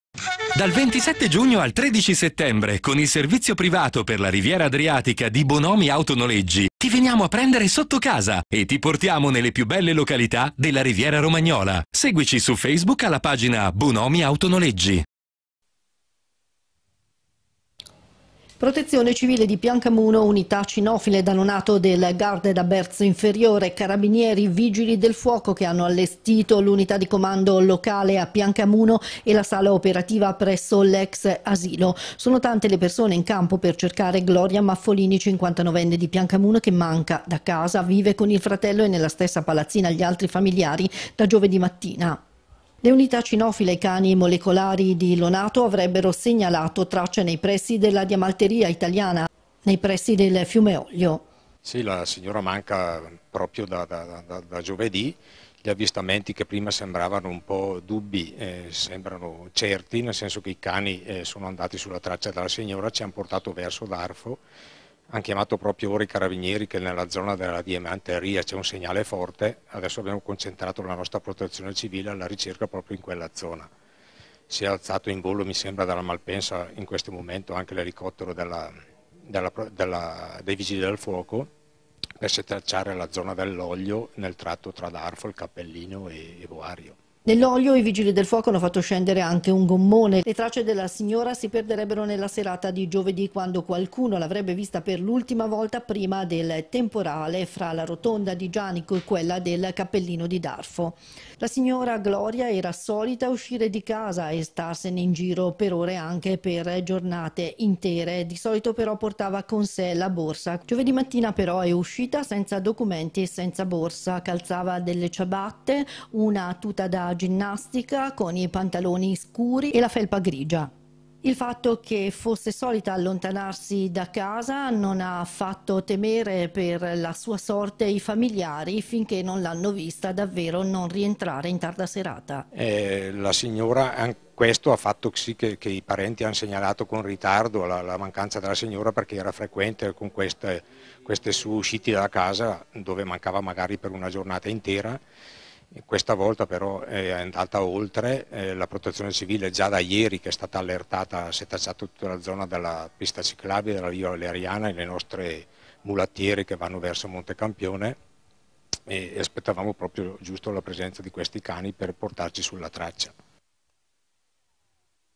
SI CERCA GLORIA MAFFOLINI
Sono cominciate nella giornata di ieri e sono proseguite per tutta la domenica le ricerche che si sono allargate al comune di Darfo di Gloria Maffolini, la 59enne scomparsa dalla sua abitazione di Piancamuno giovedì mattina.